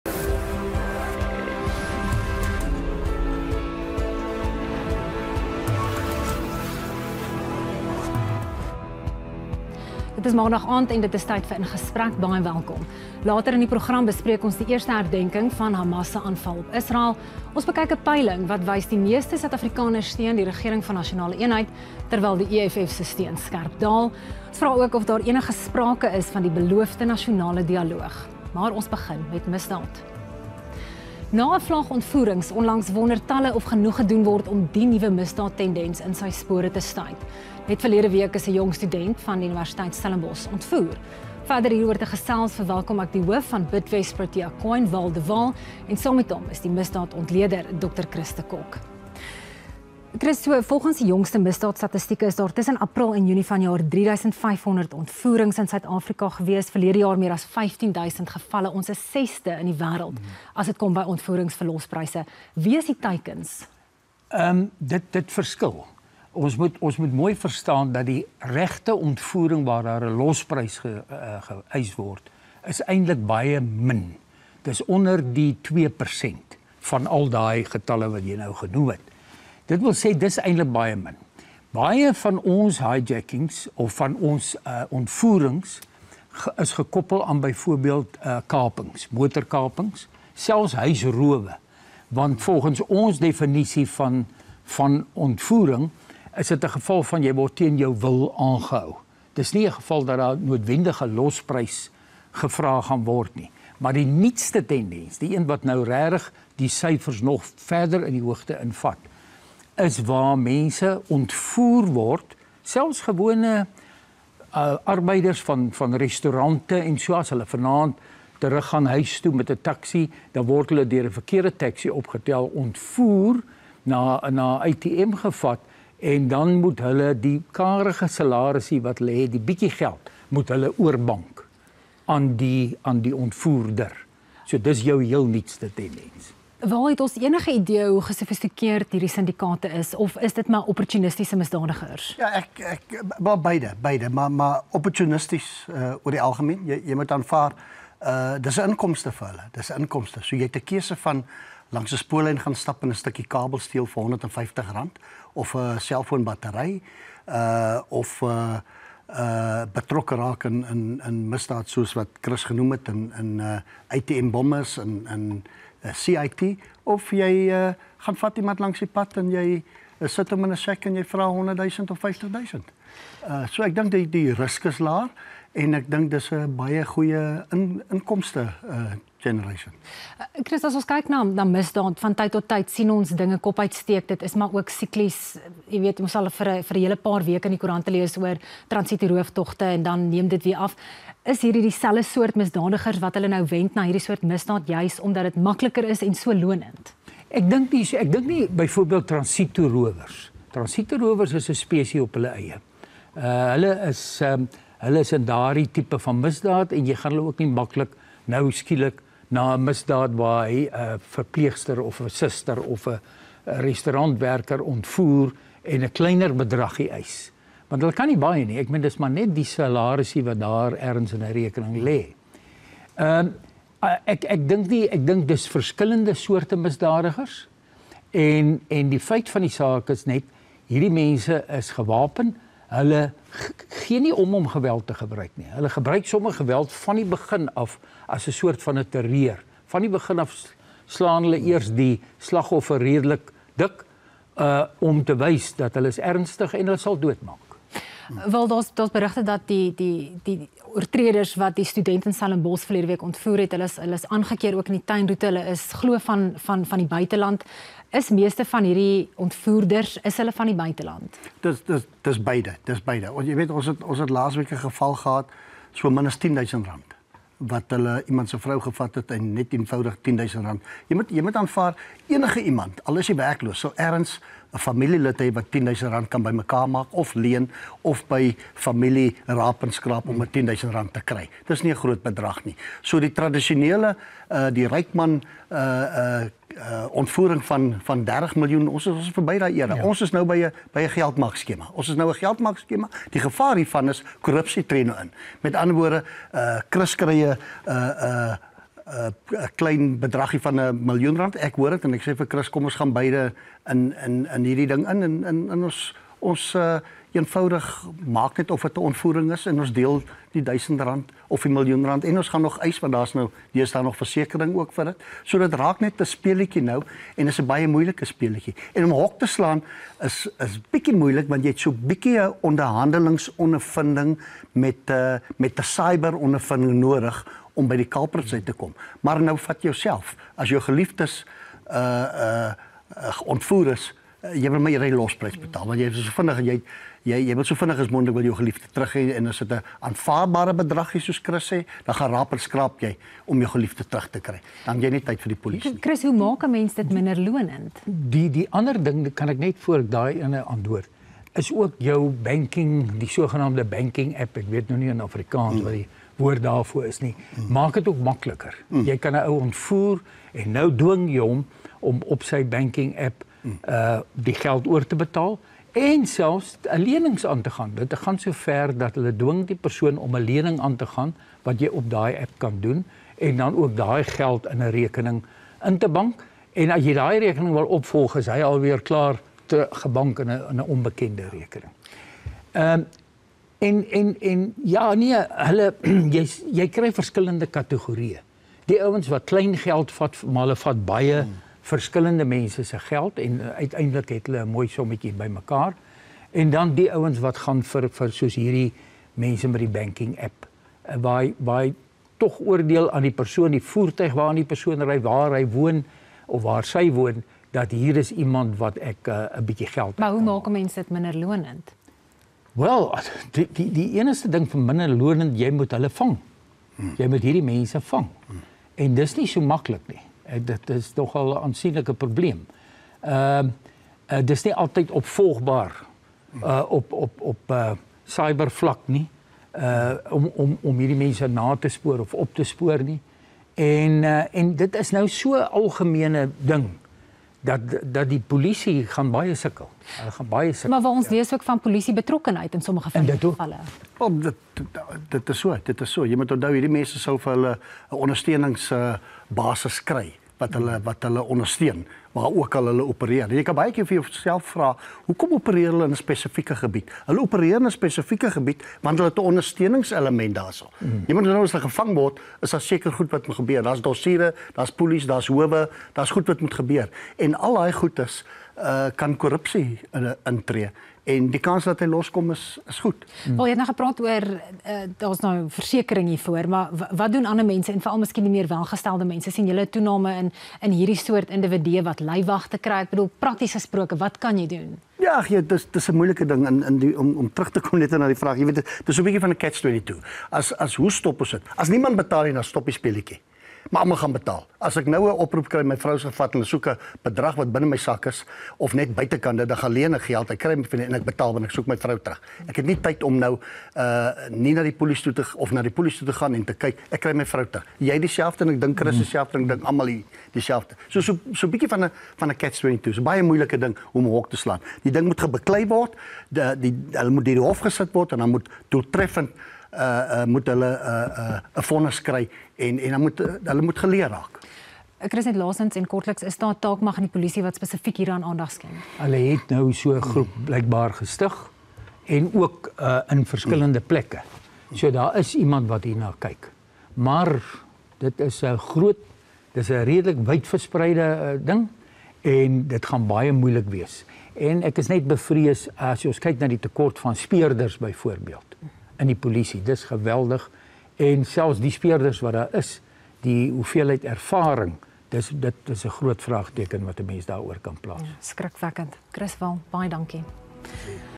Paneelgesprek met Chris de Kock en Waal de Waal | kykNET | 7 Oktober 2024
Lourensa gesels met Chris de Kock en Waal de Waal oor die kommerwekkende toename in ontvoerings oor die land heen.

Interview with Chris de Kock and Waal de Waal | kykNET | 7 October 2024
Lourensa talks to Chris de Kock and Waal de Waal about the alarming increase in kidnappings across the country